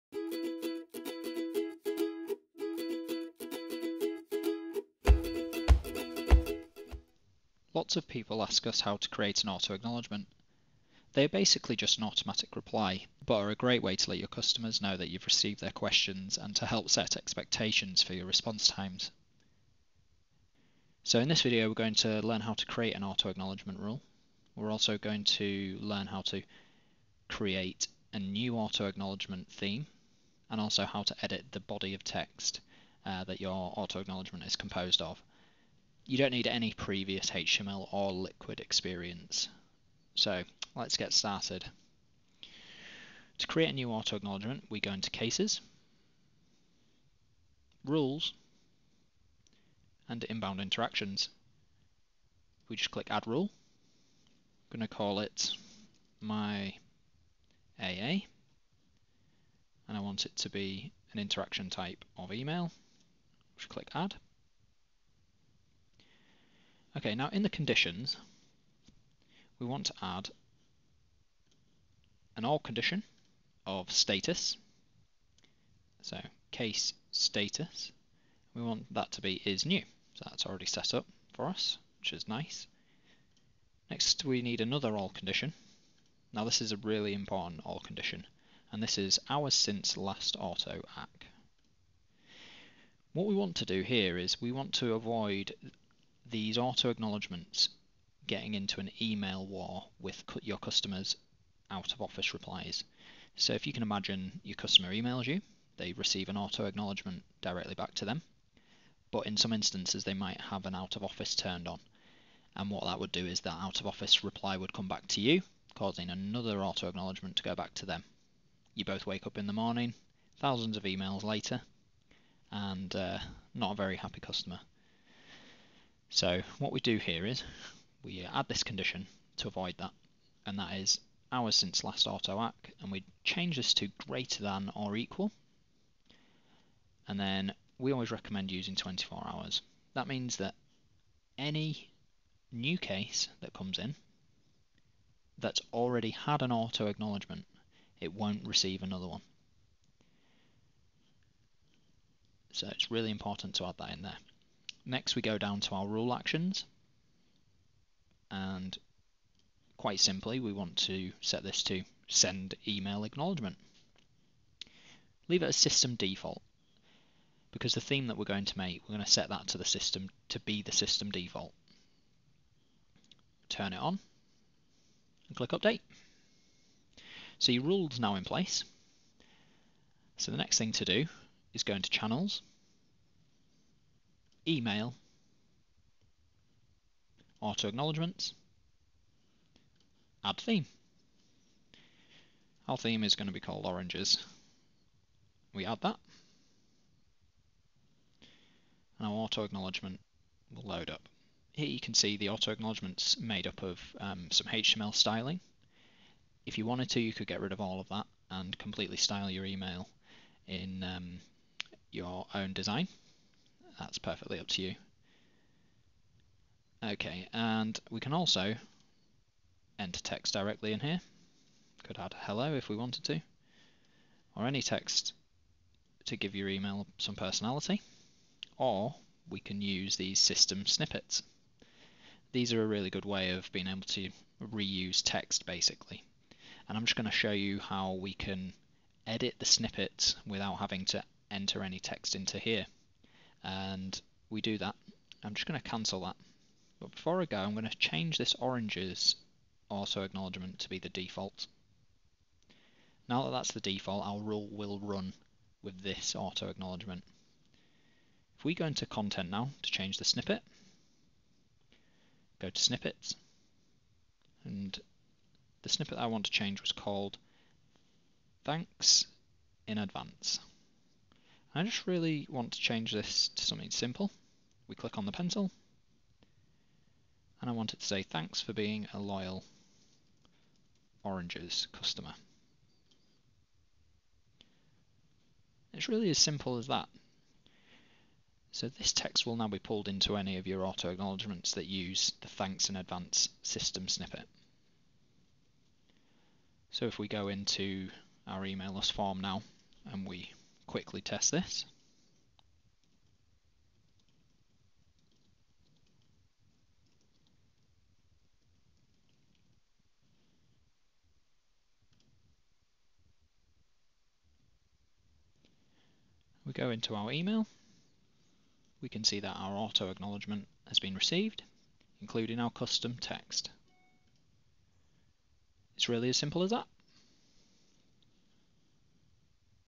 Create your own Auto-Acknowledgements using Business Rules and Snippets
Create your own Auto-Acknowledgements using Business Rules and Snippets! Allow customers to know that your have received their ticket, automatically.